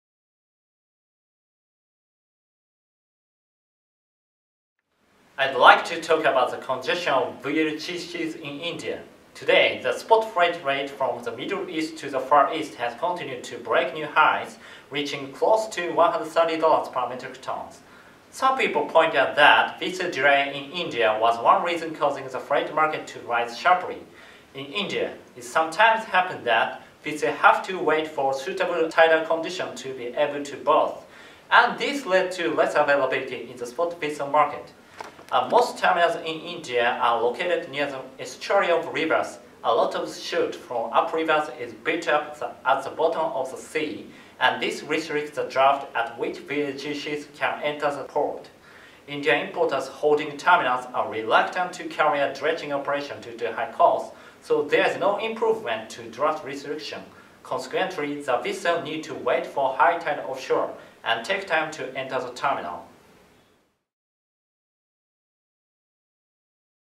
RIM Video LPG: Vessel Congestion in India
RIM LPG Reporter Shiga explains about Vessel congetions of VLGC in India. (Recorded Apr 25)